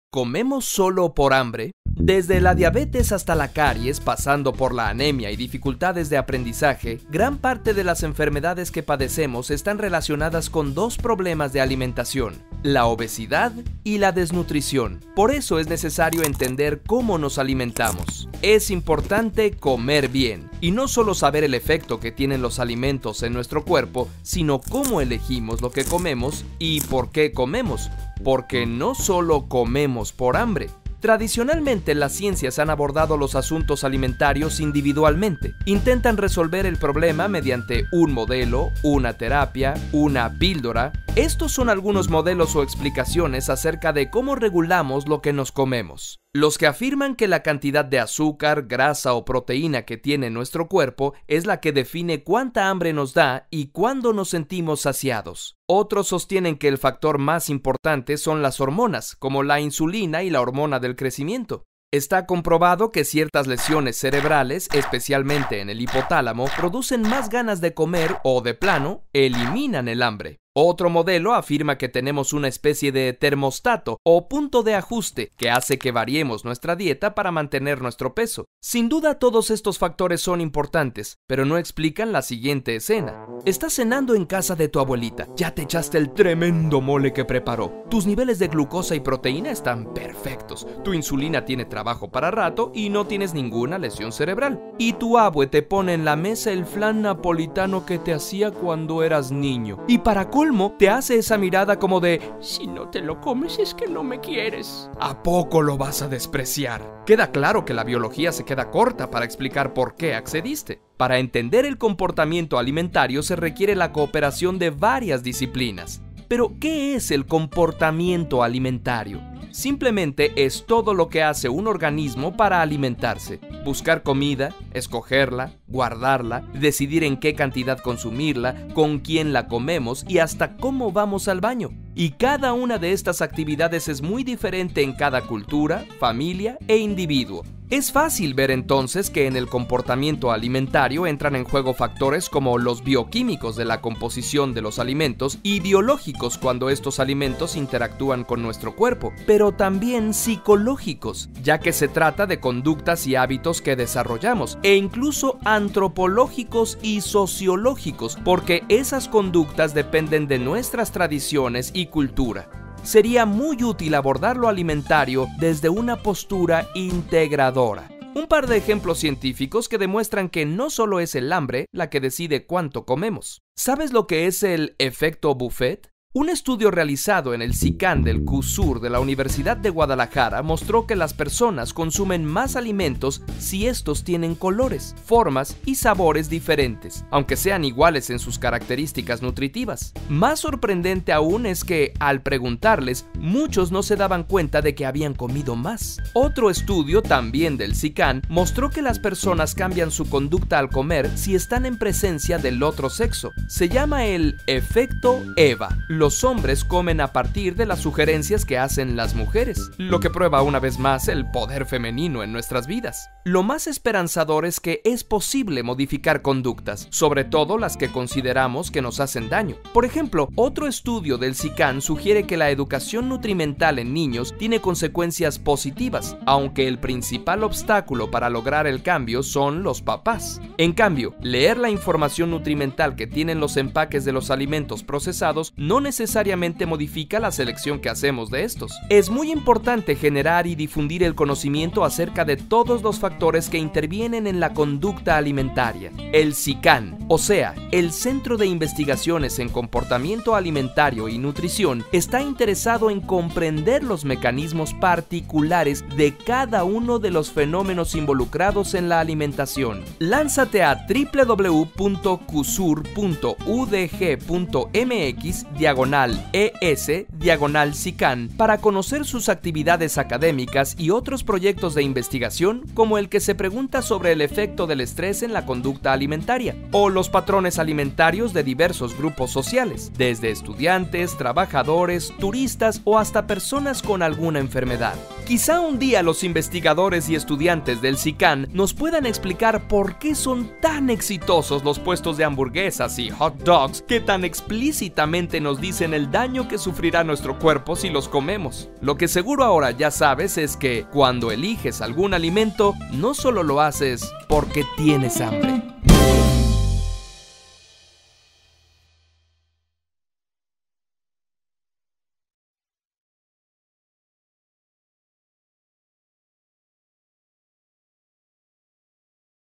¿Comemos sólo por hambre?
¿Qué es el comportamiento alimentario? El estudio de la nutrición humana suele concentrarse en los aspectos fisiológicos, pero hay muchos más que influyen en por qué y cómo comemos lo que comemos.